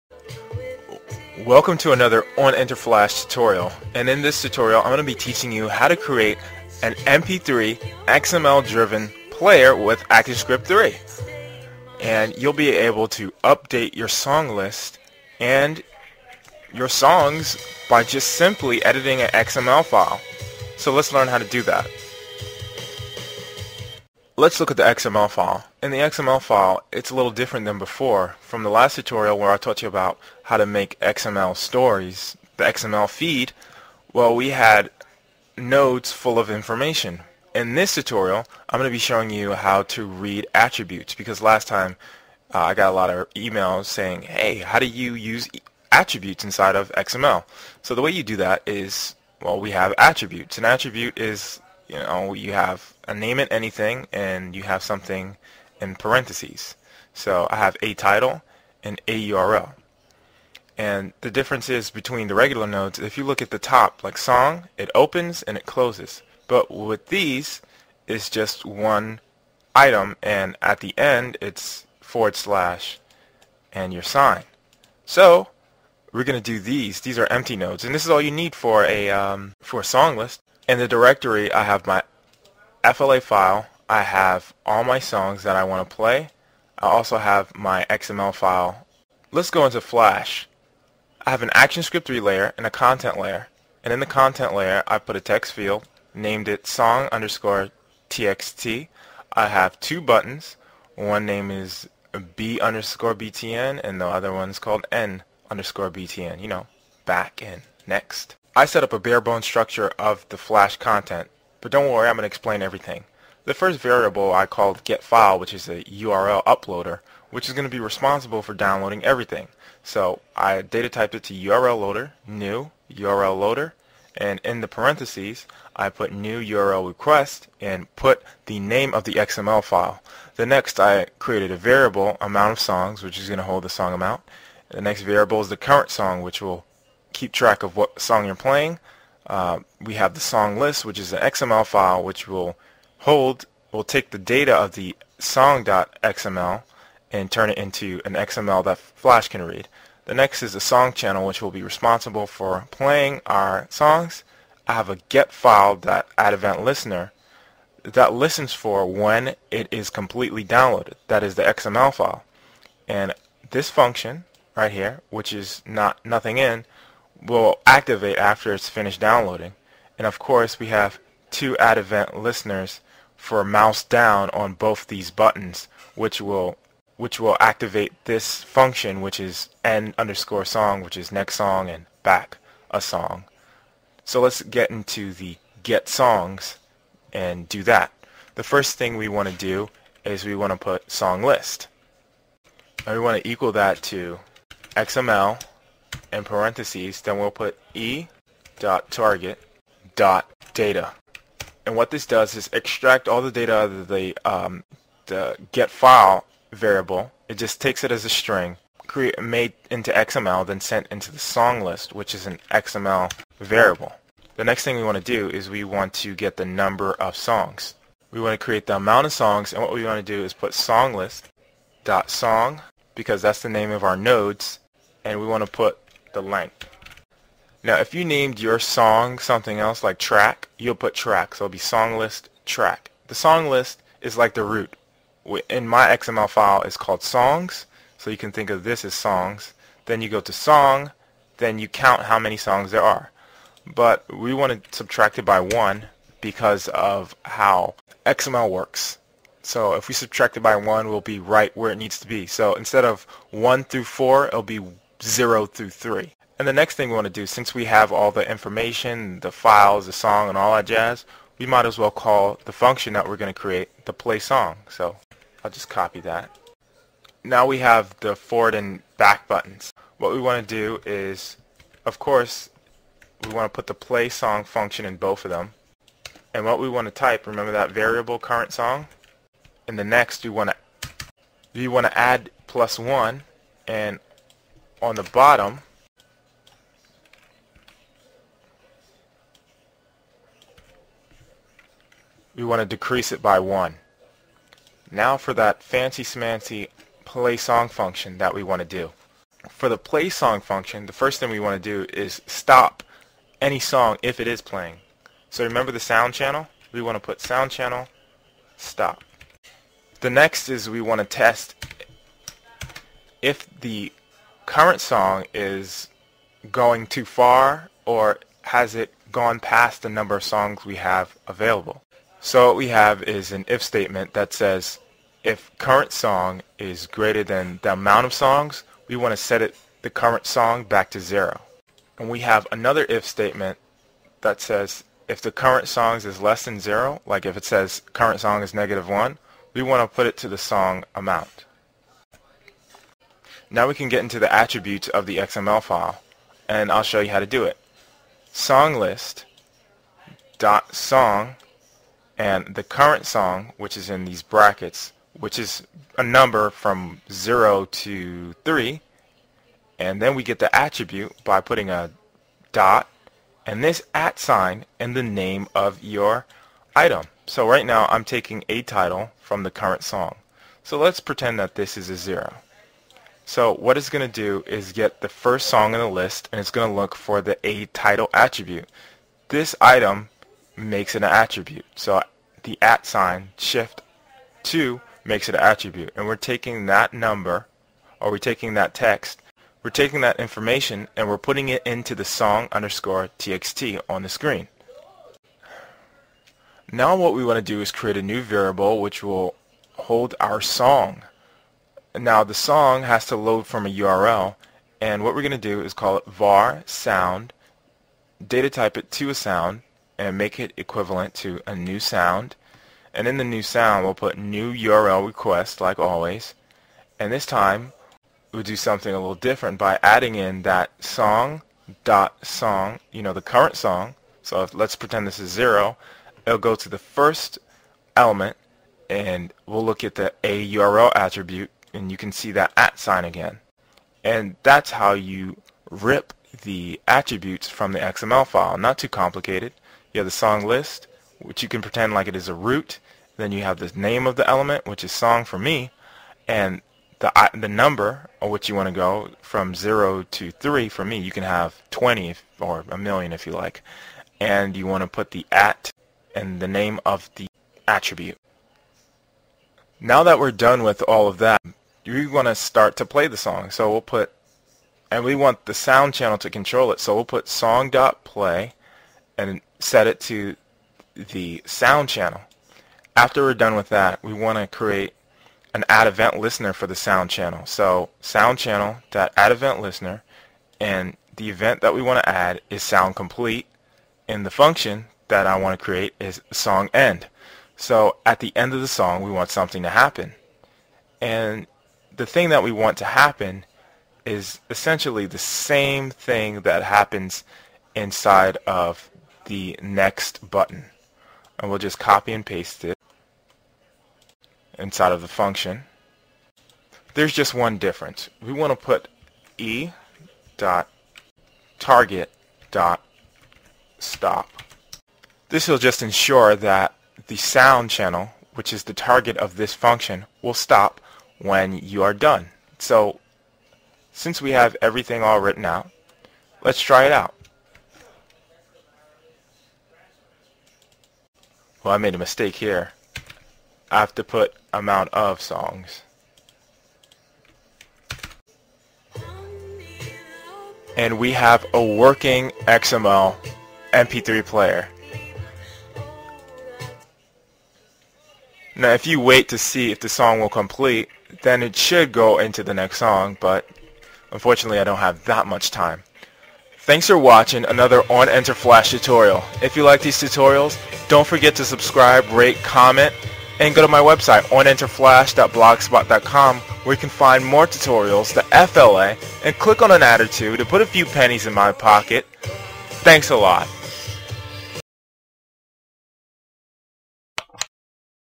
Adobe flash cs4 website tutorial: 106 XML MP3 Player
Learn how to create an XML driven mp3 player using actionscript 3 and adobe flash cs4. This is great for websites that want to play a tune while the user explores their website.